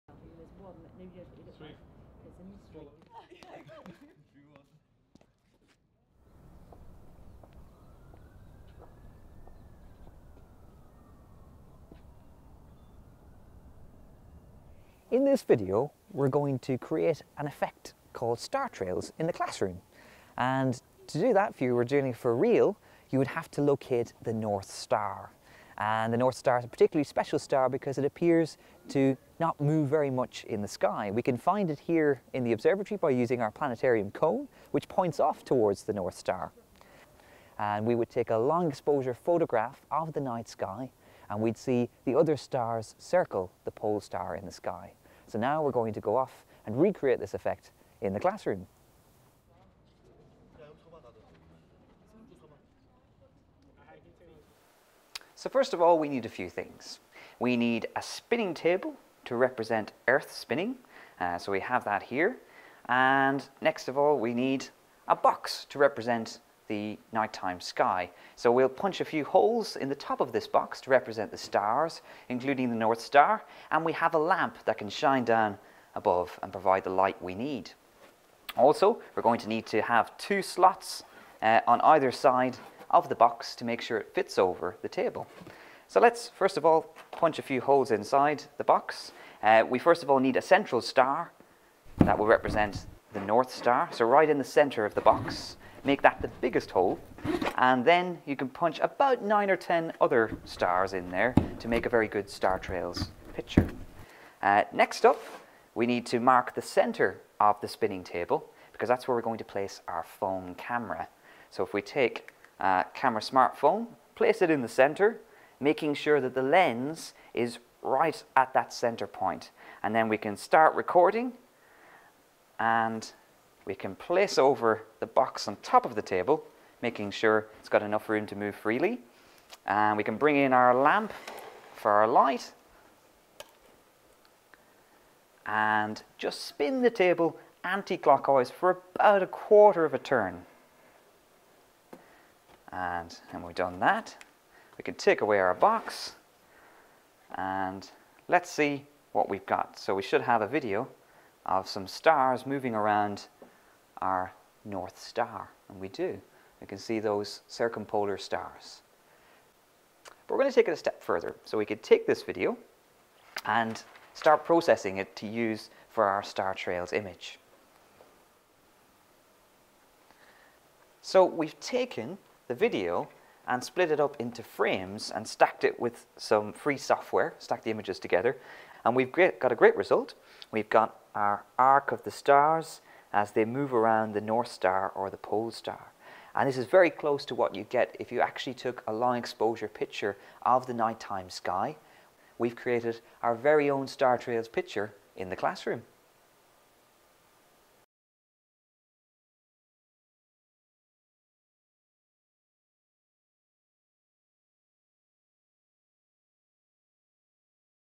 Making star trails
Royal Observatory astronomer Brendan simulates a photographic effect known as 'star trails'. The same computer processing techniques used to obtain the final image in the video can be used for capturing images of a real night sky. (KS4 and post-16)